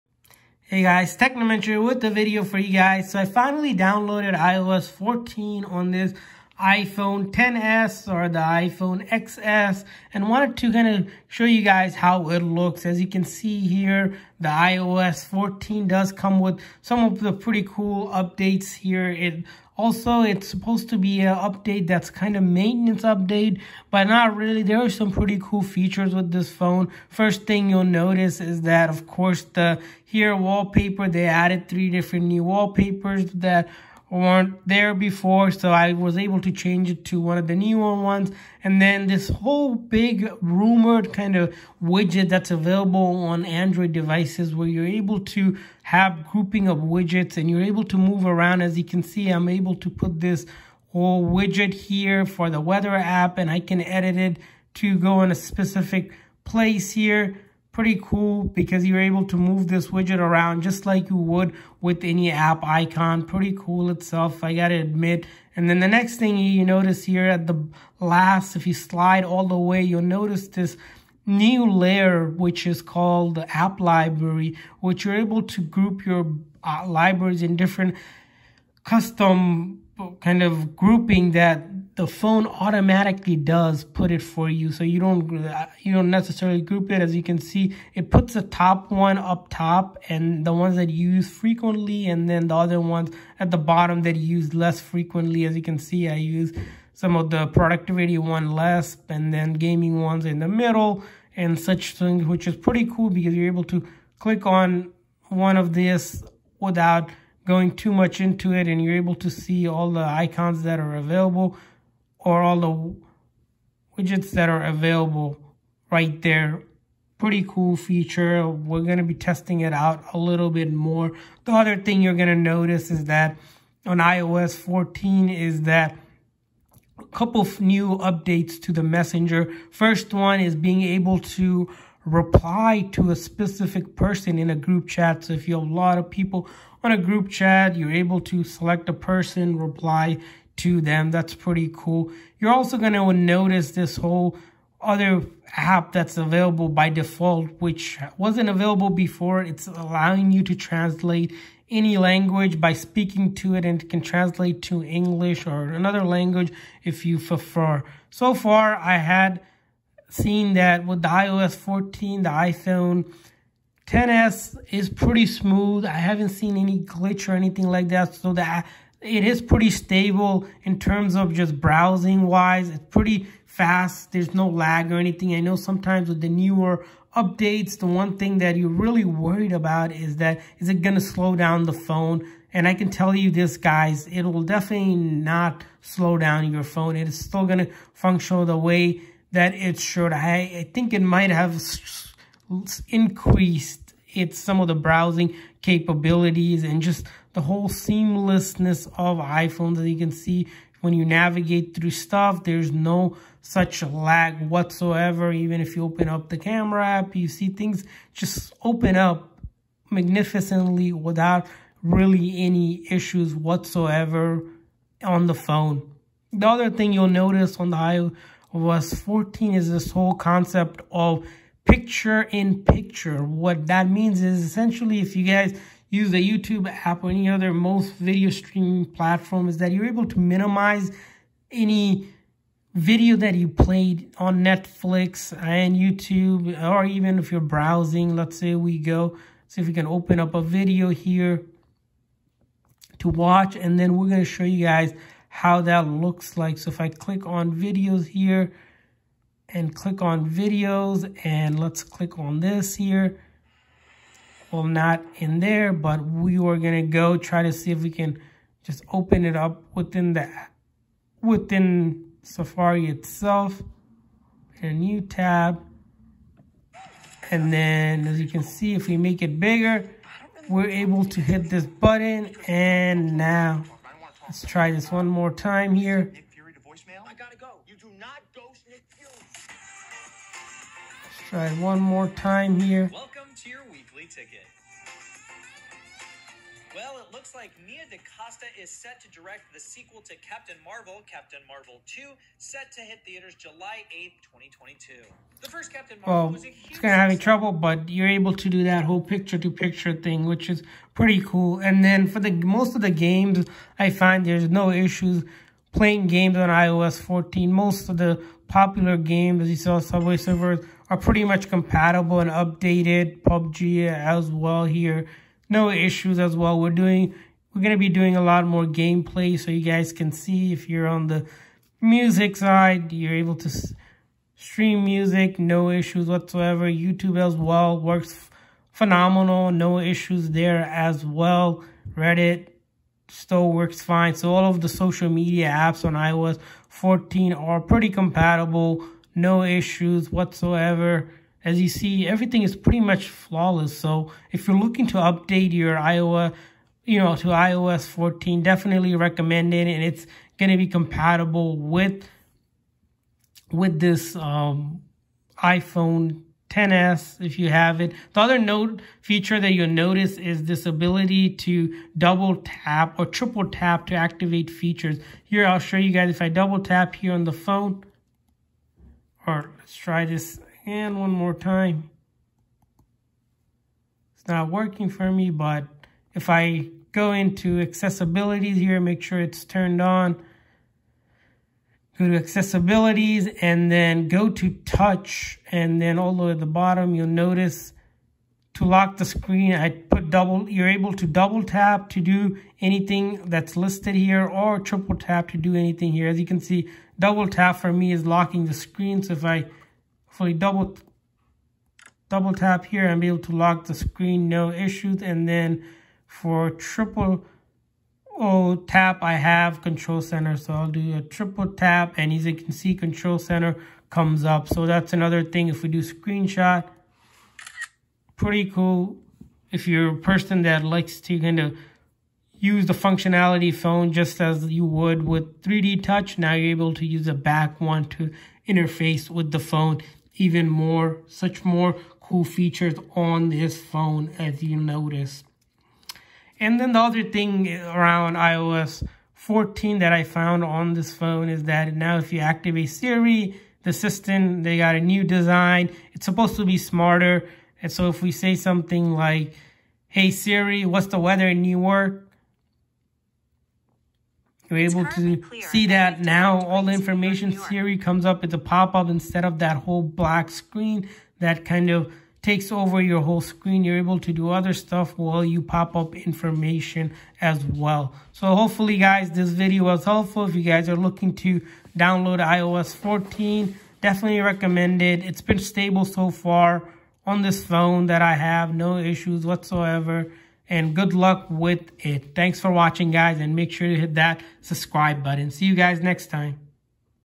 iOS 14 OFFICIAL on iPhone XS
In this video, we take a look at the latest iOS 14 update on the iPhone XS. We explain all you need to know before updating to iOS 14 on your Apple iPhone Xs.